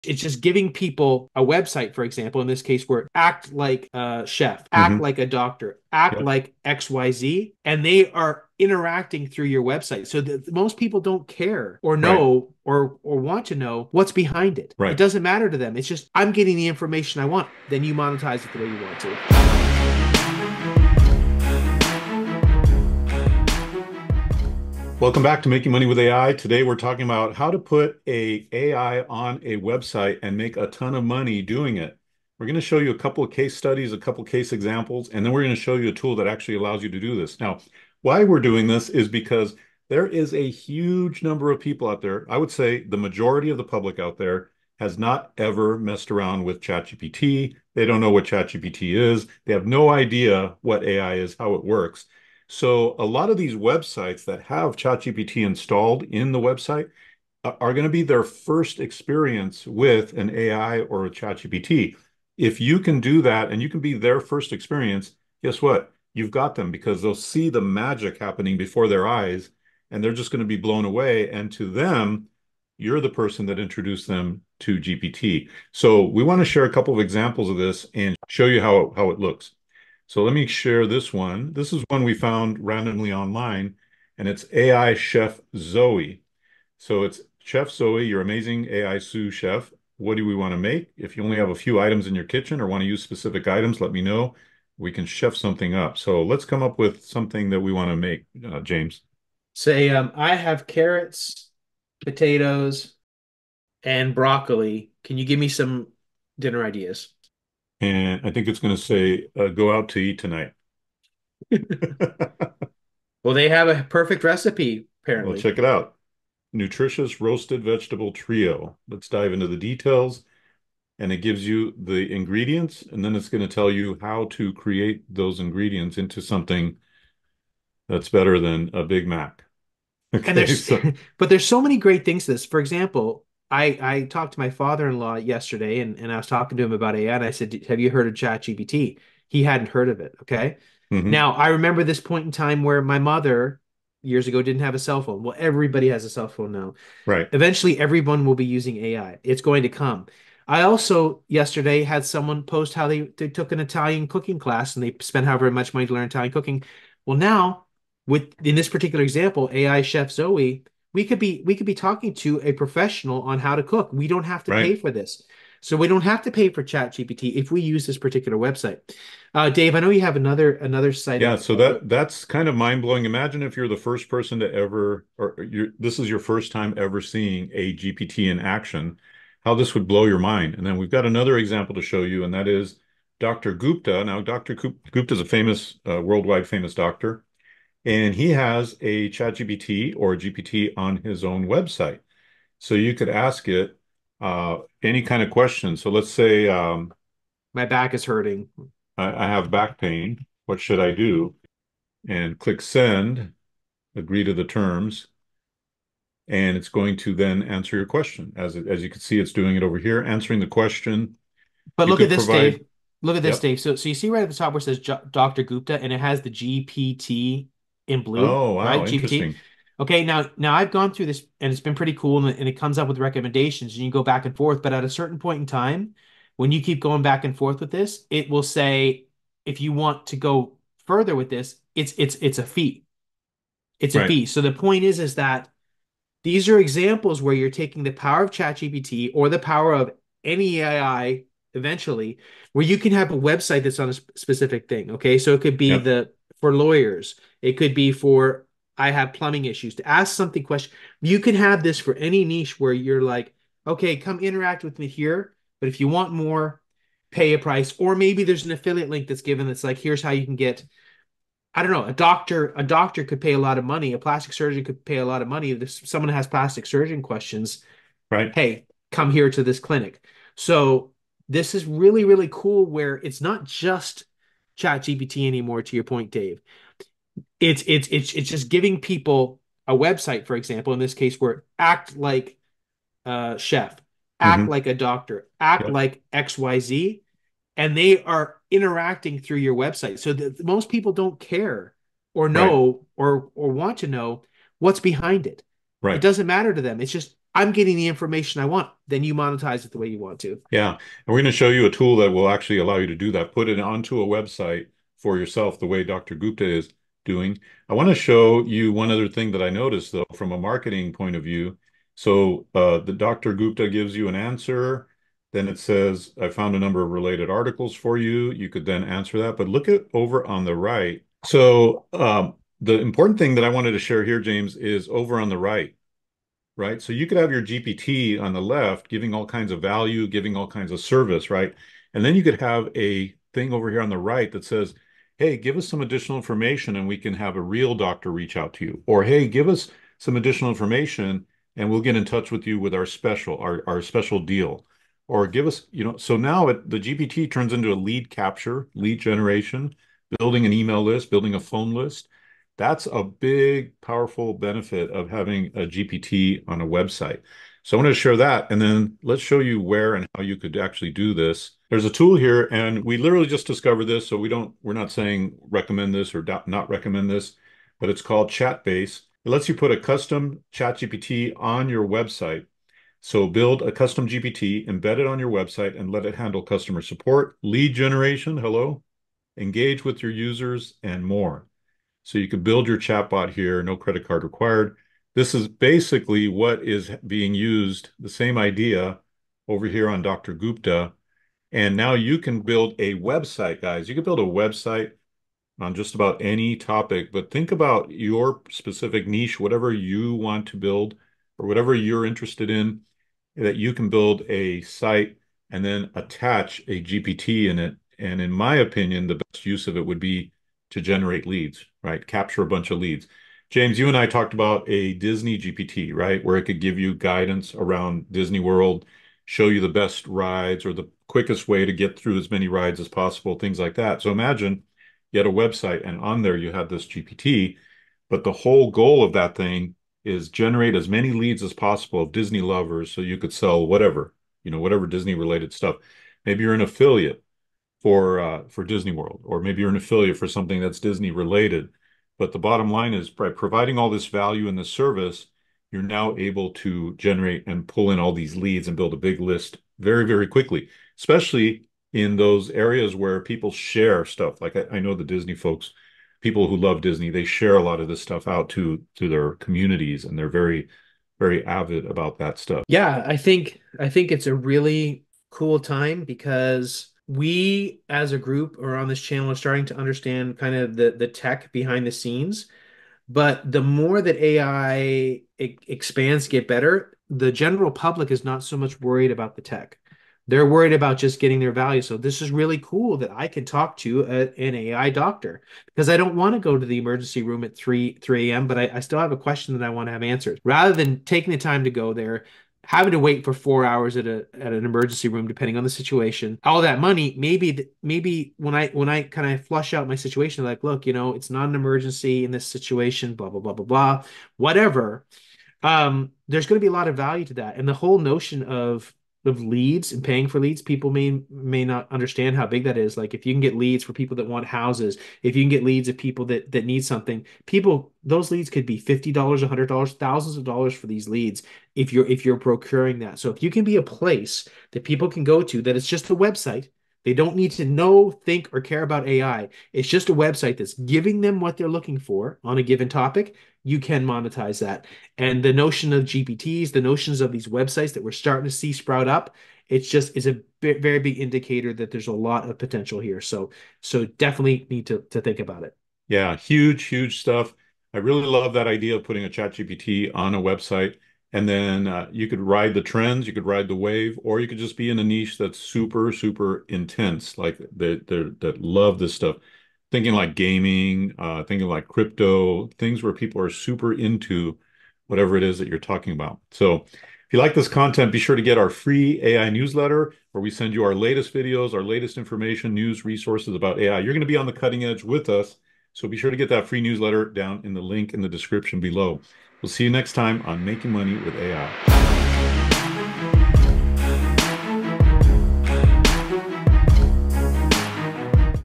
Make THOUSANDS Installing AI ChatBots On Websites!
Make Money Online
Make Money Online With AI

🚀 Dive into the world of AI-powered websites in this eye-opening episode!

We're exploring how to implement AI, specifically ChatGPT, on websites to revolutionize customer interaction and lead generation. 🤖💡

🔥 Watch as we showcase case studies and tools that allow you to embed custom ChatGPT into your site, transforming it into an interactive, value-adding platform.

Whether you're a fan of AI Chef Zoey or Dr. Gupta, learn how to make AI work for you!